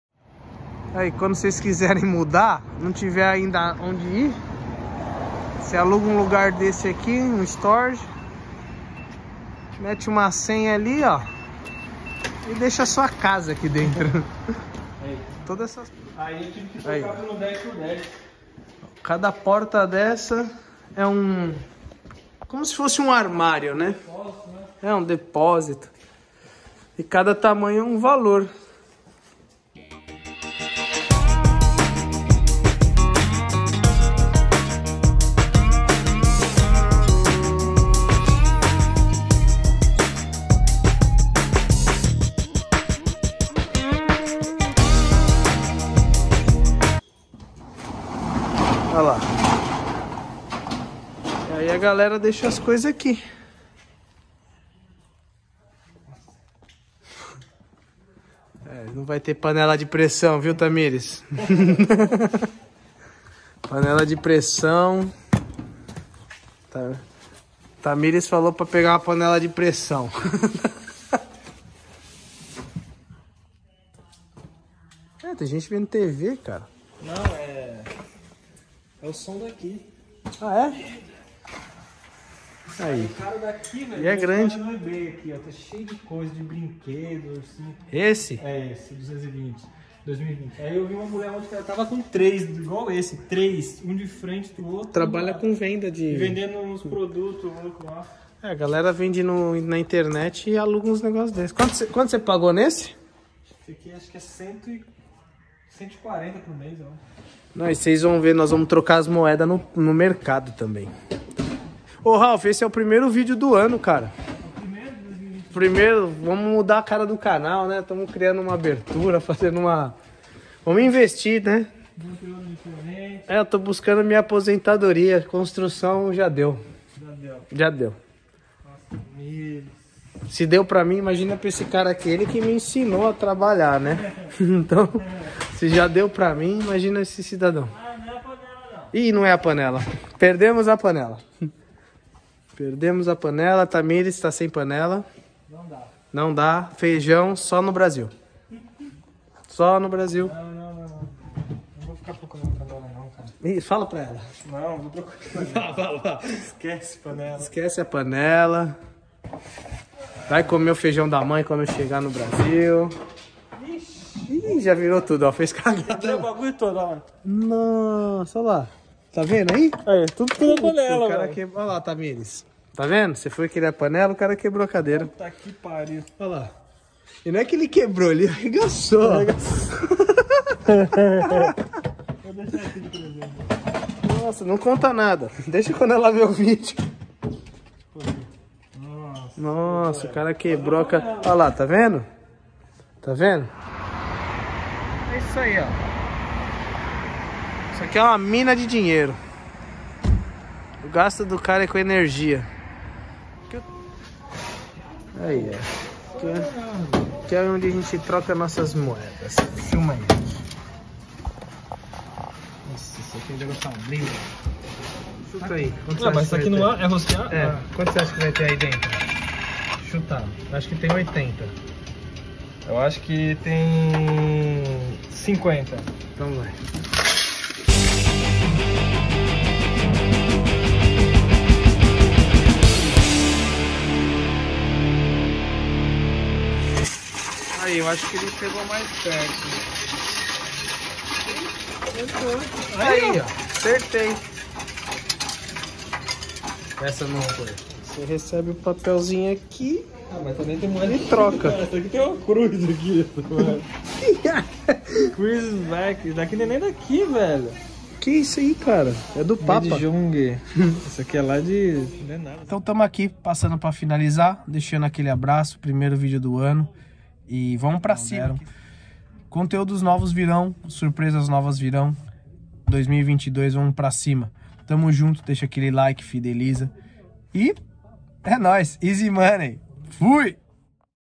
COMO FUNCIONA UM STORAGE NOS EUA!!!
Meu cunhado esta tirando umaS ferias, e alugou um Storage para deixar suas coisas! Como trocamos moedas aqui nos EUA.

Rodrigo araujo